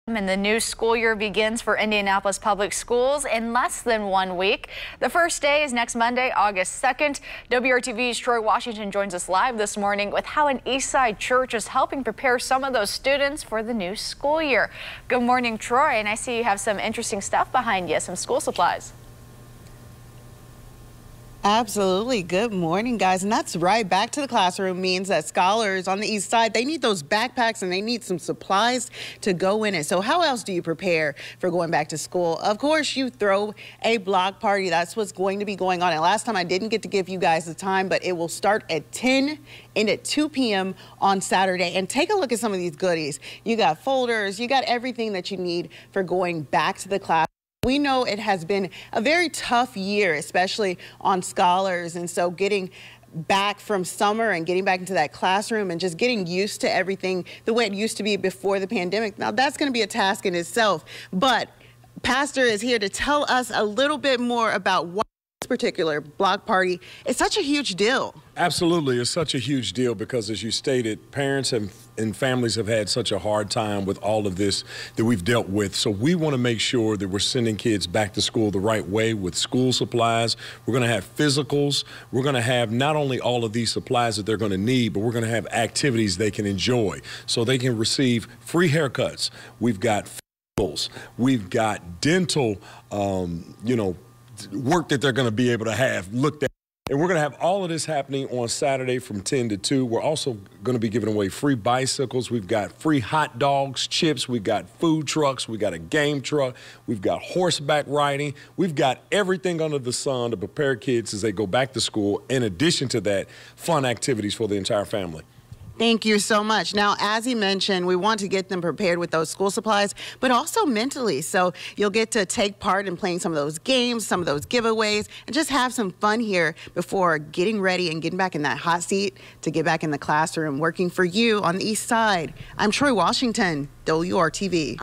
Back-to-school block party preparing students for success
WRTV's Troy Washington is live at a back-to-school block party for students.